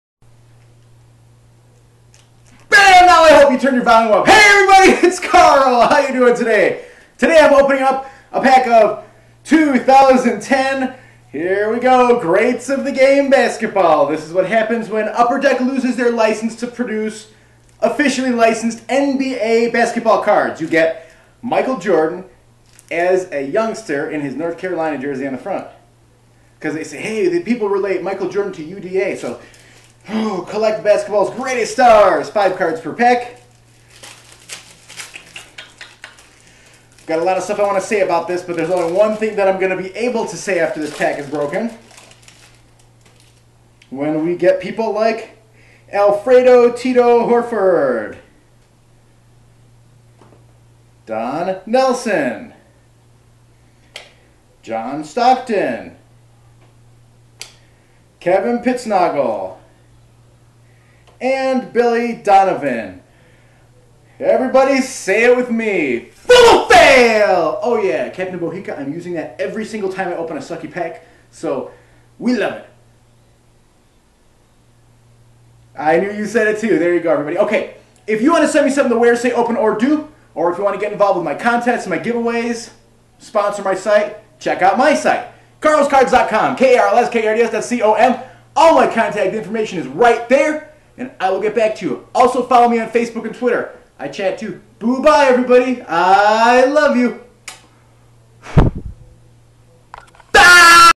2010 Upper Deck Greats of the Game
Today I opened a pack of failure - check it out.

If you'd like information on how to enter my contests, please visit my